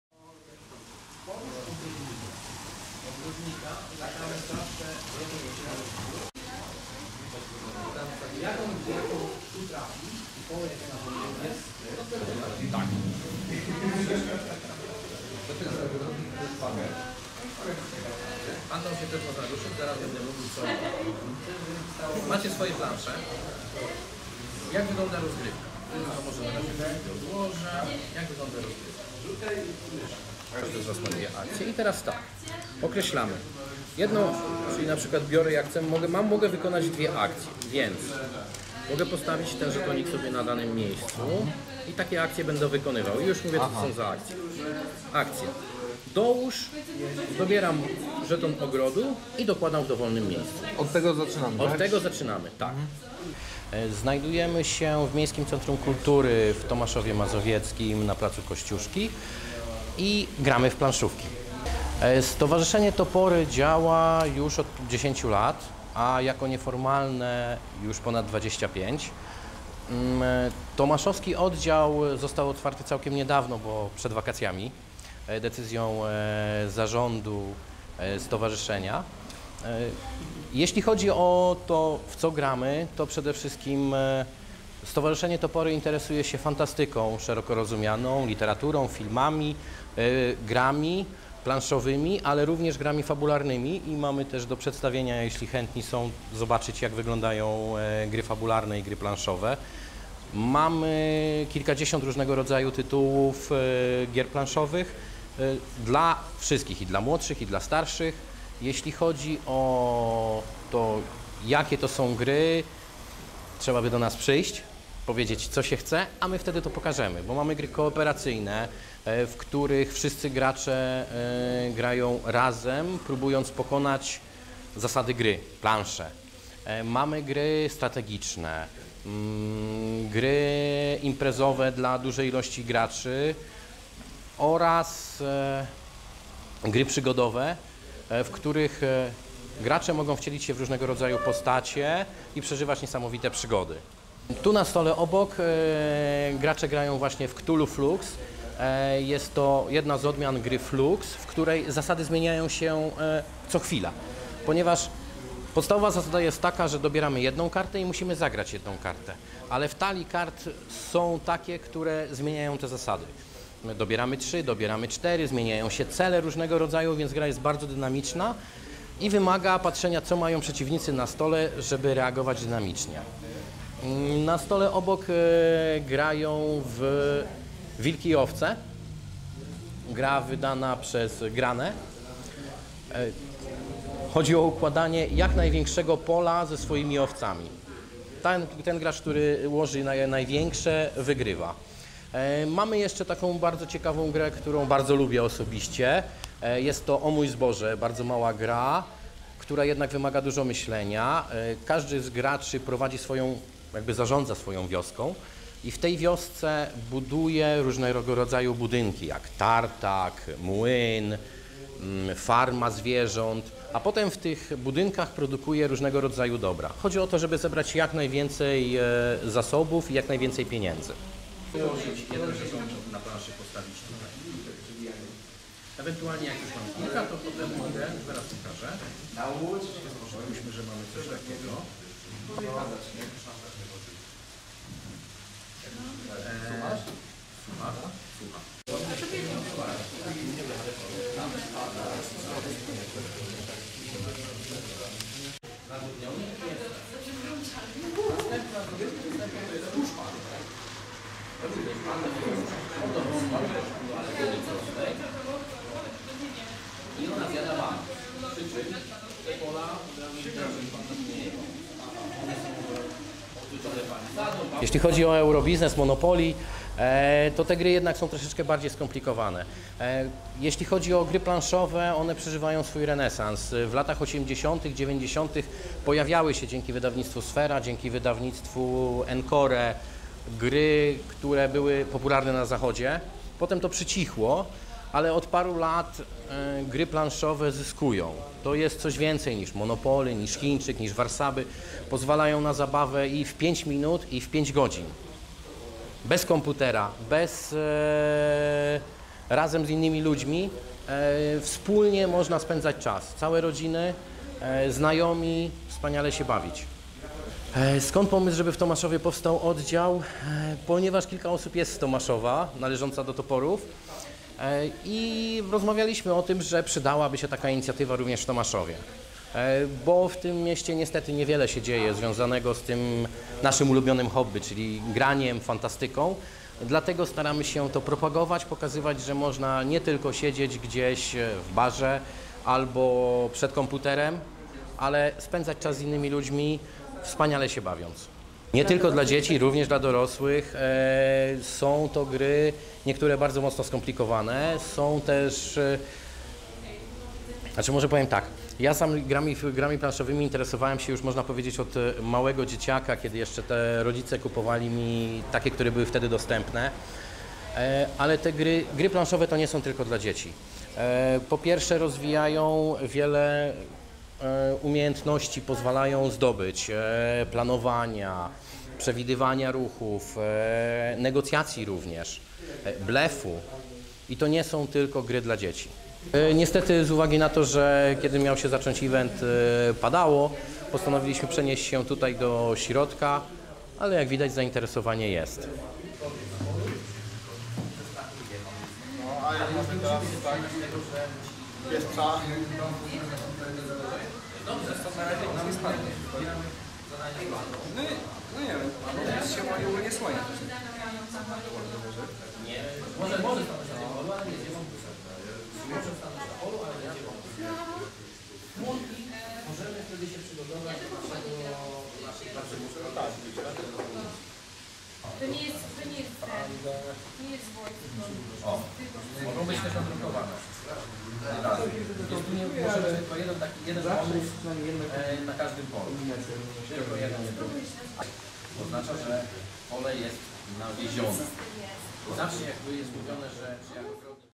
„Kulturalne piątki” - Gry planszowe - zabawa z planszówką
Kolejny "Kulturalny piątek na placu Kościuszki w Tomaszowie Mazowieckim. Tym razem Miejskie centrum Kultury zaprosiło do organizacji zajęć tomaszowski oddział Rawskiego Stowarzyszenia  Miłośników Gier Fabularnych i Fantasy "Topory".
Na rozpoczęcie zabaw z grami planszowymi rozpadał się deszcz i organizatorzy przenieśli się do pomieszczeń Miejskiego Centrum Kultury. Królowały gry planszowe. Uczestnicy, wśród których były dzieci i ich rodzice przy stolikach poznawali zasady gier które proponował Sebastian Wawrzyńczyk, prezes tomaszowskiego oddziału Stowarzyszenia "Topory".
"Tomaszowski oddział został otwarty niedawno, bo dopiero przed wakacjami decyzja Zarządu Stowarzyszenia. Jeśli chodzi o to, w co gramy, to przede wszystkim Stowarzyszenie "Topory" interesuje się fantastyką szeroko rozumianą, literaturą filmową, grami planszowymi, filmami, ale również grami fabularnymi. i mamy również do przedstawienia, jeśli chętni są, zobaczyć jak wyglądają gry fabularne i gry planszowe. Mamy kilkadziesiąt różnego rodzaju tytułów gier planszowych dla wszystkich. Dla młodszych i dla starszych. Mamy gry kooperacyjne, w których wszyscy gracze grają razem, próbując pokonać zasady gry, plansze. Mamy gry strategiczne, gry imprezowe dla dużej ilości graczy oraz gry przygodowe, w których gracze mogą wcielić się w różnego rodzaju postacie i przeżywać niesamowite przygody." - powiedział ciekawie o grach, jakie posiada oddział tomaszowski Stowarzyszenia "Topory".